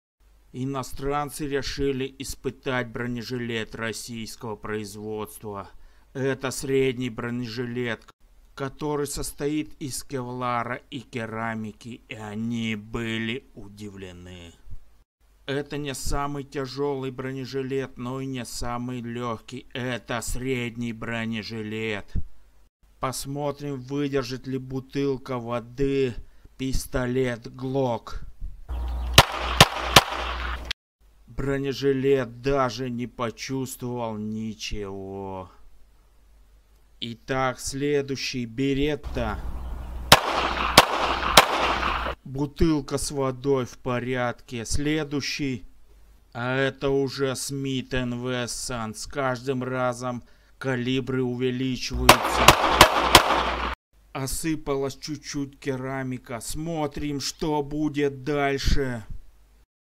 Российский бронежилет.ч1.Удивил иностранцев.
Иностранцы решили испытать средний бронежилет Российского производства и были удивлены.Испытание начали проводить с маленьких пистолетов и заканчивалось испытанием винтовками.Бронежилет состоит из кевларавых пластин и и керамики.
 В первой части проходят испытания пистолеты Glock , Beretta и Smith & Wesson .